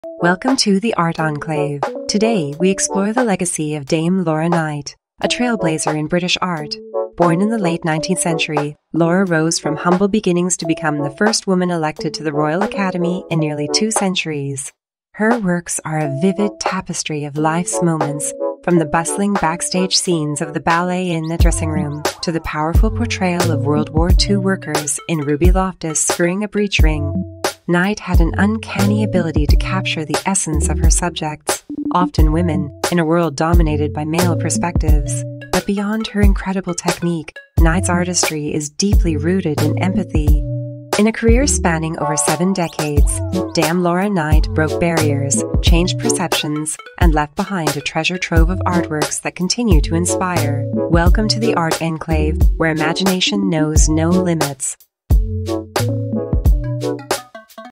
Breaking Brushes: The Bold Legacy of Dame Laura Knight
Journey into the vibrant world of Dame Laura Knight, a trailblazing British artist who shattered barriers and captured the essence of her times. From evocative portraits to mesmerizing landscapes, discover the genius behind the canvas and the legacy she imprinted on the art world. Join us on 'The Art Enclave' as we delve deep into the colorful tapestry of Knight's life and masterpieces.